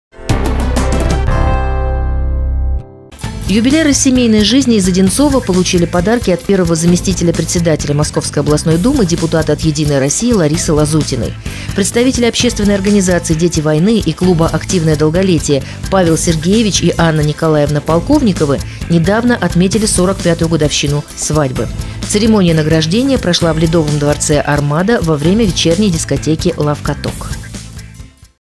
Юбиляры семейной жизни получили подарки от Ларисы Лазутиной
Представители общественной организации «Дети войны» и клуба «Активное долголетие» Павел Сергеевич и Анна Николаевна Полковниковы недавно отметили 45-ю годовщину свадьбы.